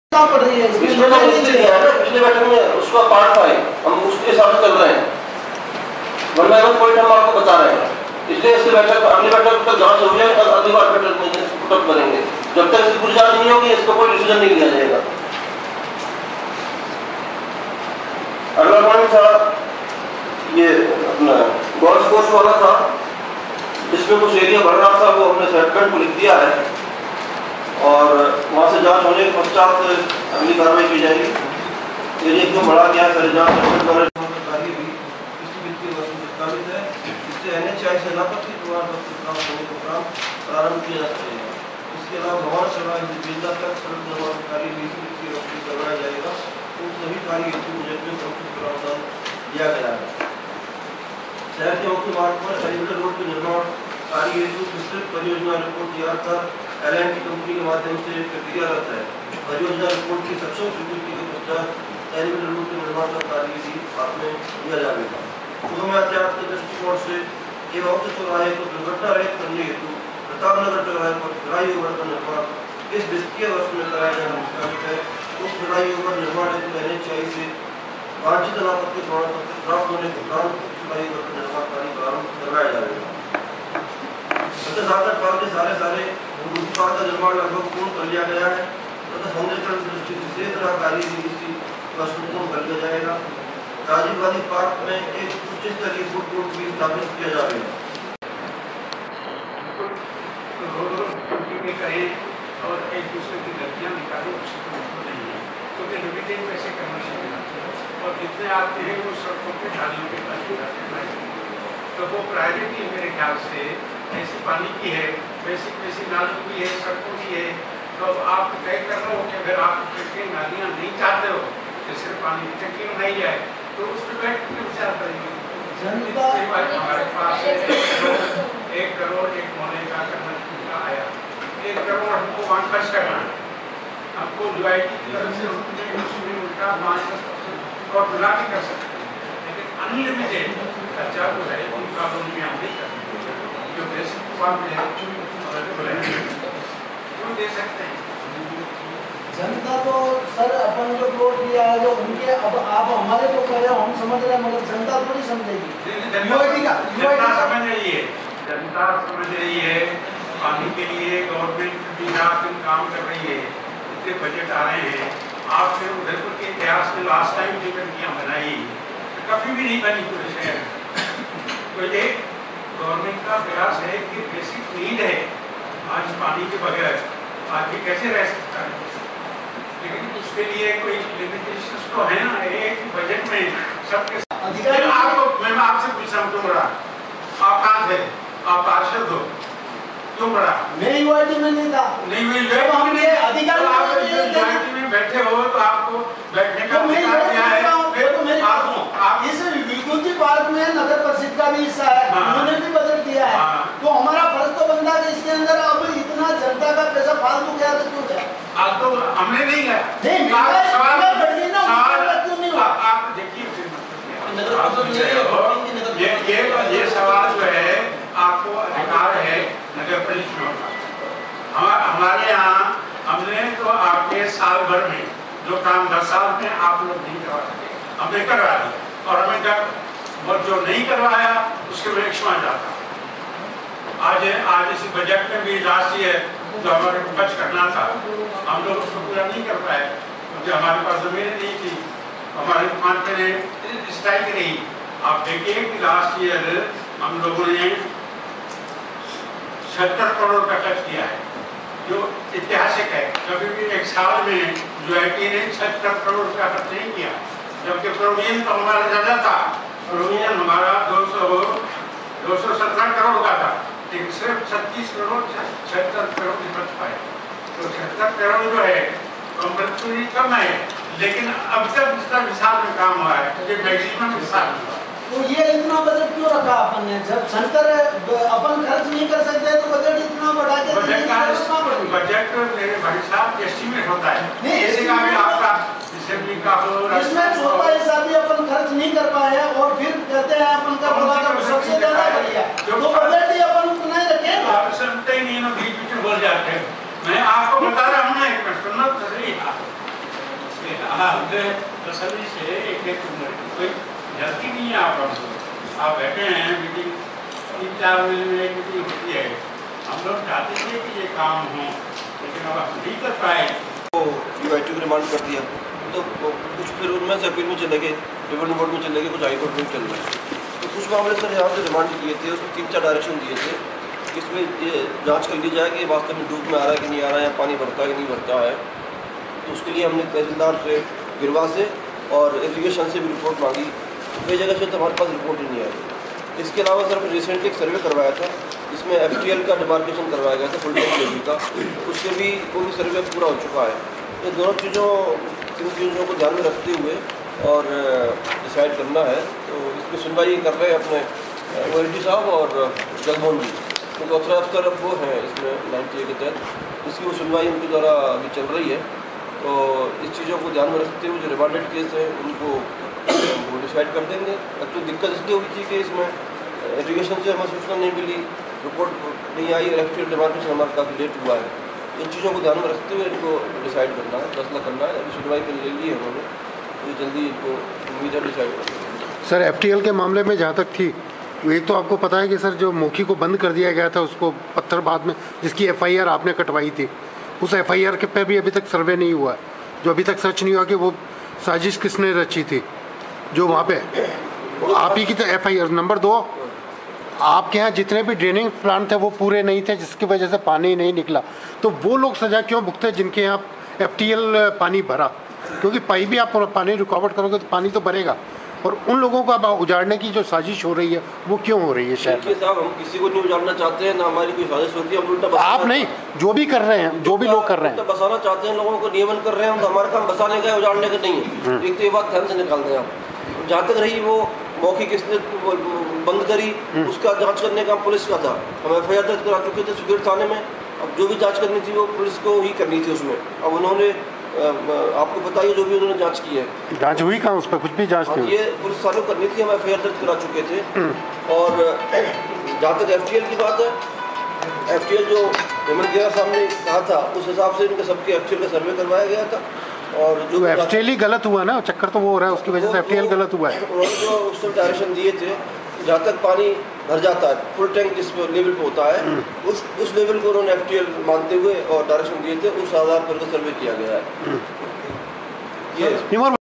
UIT budget for the year 13-14 passed
Udaipur, Annual budget of UIT Udaipur, has passed in a meeting held inn UIT hall. Secretary Dr, RP Sharma presented the budget. Senior accounts officer BP Sharma has preapared the budget. Chairman Roop kumar Khurana said that UIT is committed to provide tne best aminities to citizens.